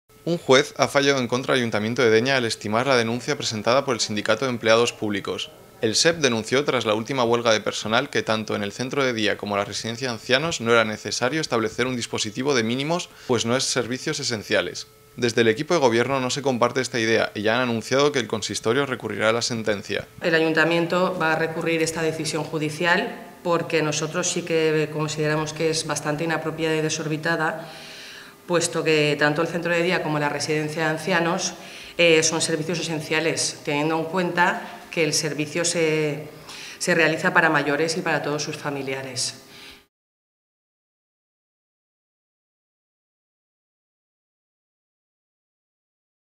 El ayuntamiento recurre la sentencia que reprueba la apertura del Centro de Día de Santa Llúcia en
Más información en Tvdénia.com